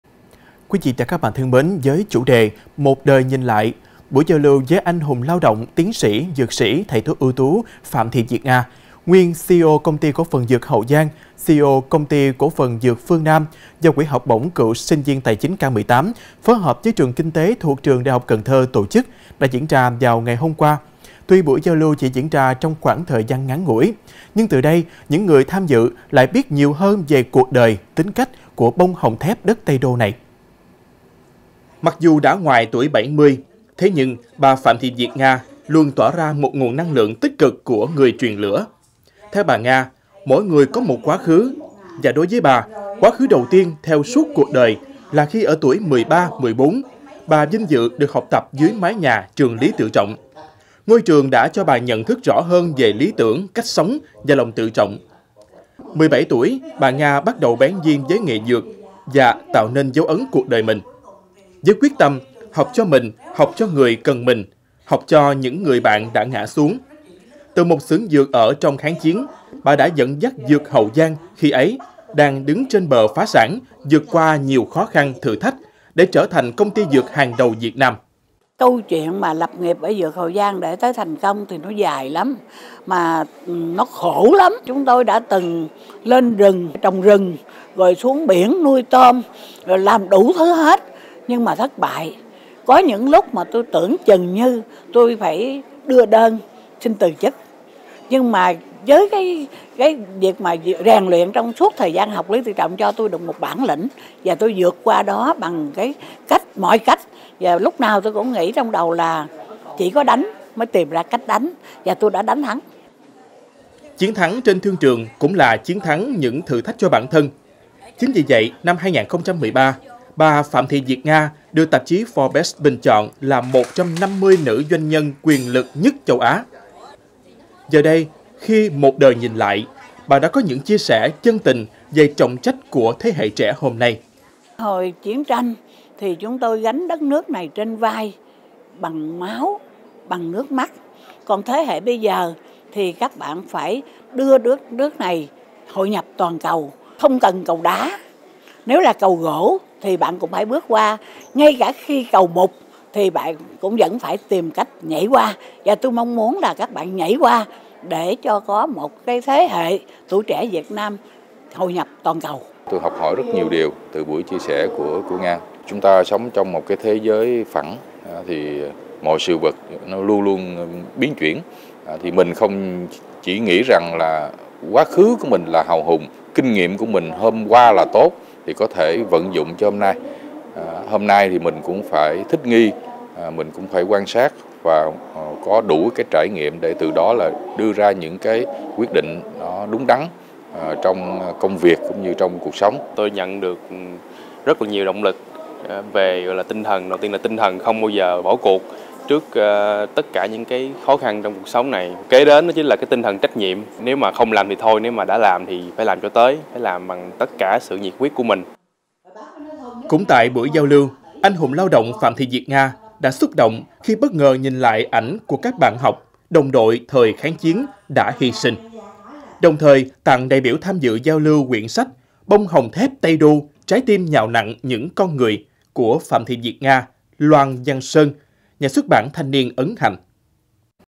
"Một đời nhìn lại" của bông hồng thép đất Tây Đô | Cần Thơ TV
Với chủ đề “Một đời nhìn lại”, buổi giao lưu với Anh hùng Lao động, Tiến sĩ, Dược sĩ, Thầy thuốc ưu tú Phạm Thị Việt Nga – nguyên CEO Công ty CP Dược Hậu Giang, CEO Công ty CP Dược Phương Nam do Quỹ học bổng cựu sinh viên Tài chính K18 phối hợp Trường Kinh tế thuộc Trường Đại học Cần Thơ tổ chức đã diễn ra ngày 26/11.